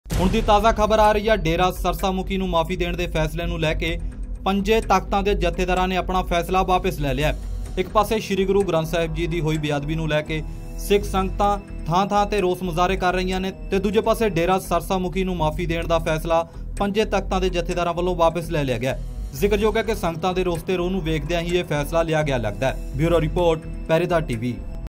dera sirsa news16-10-2015
ਡੇਰਾ ਸਿਰਸਾ ਮੁਖੀ ਨੂੰ ਦਿੱਤੀ ਮੁਆਫੀ ਦਾ ਫੈਸਲਾ ਜੱਥੇਦਾਰਾਂ ਵੱਲੋਂ ਲਿਆ ਗਿਆ ਵਾਪਿਸ